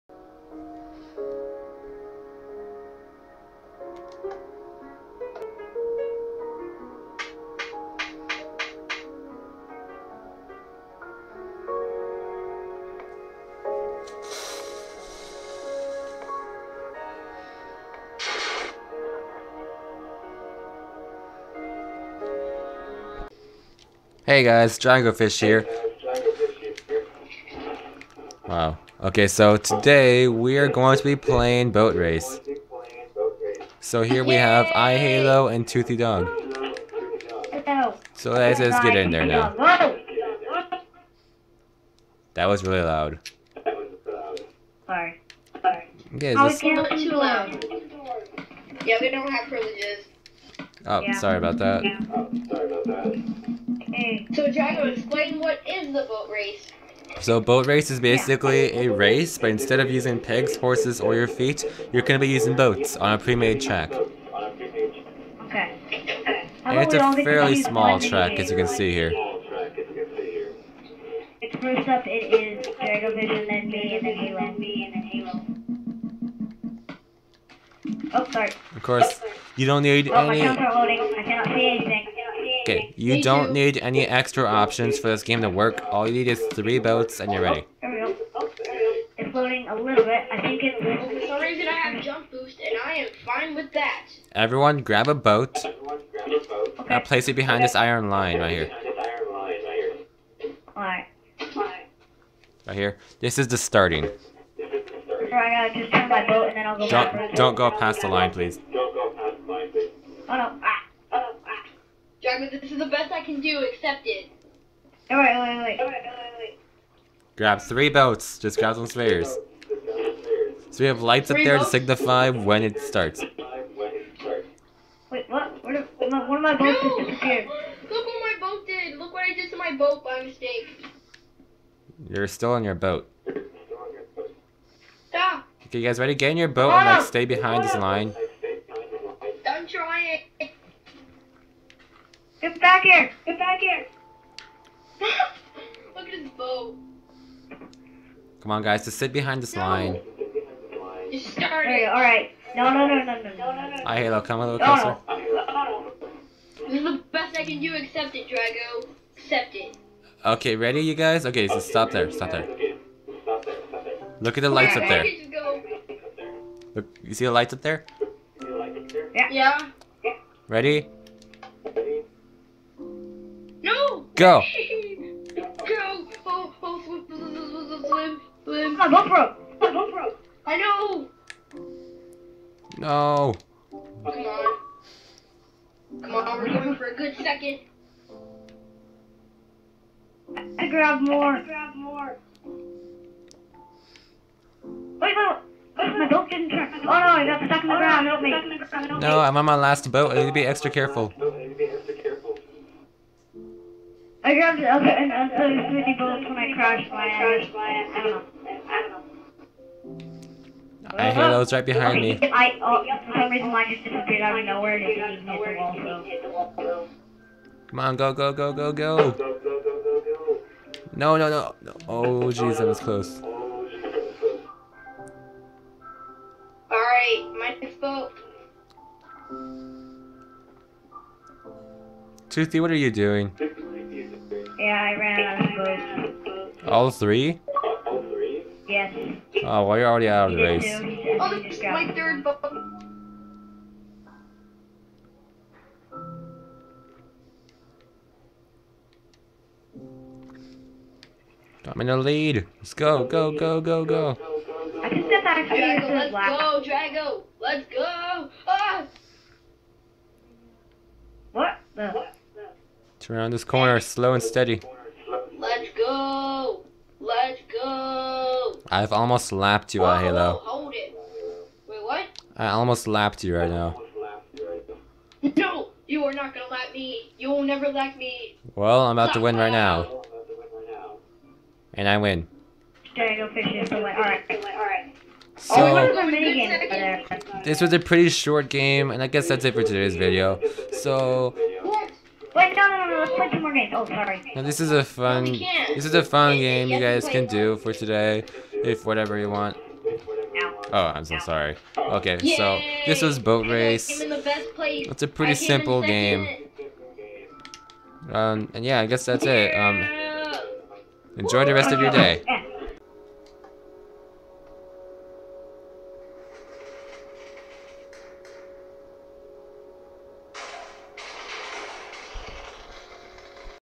Minigame Madness V3 episode 15; Boat Race
I created this video with the YouTube Video Editor Me and friends go onto our boats and try to race through a custom race track. Who will win in this challenge.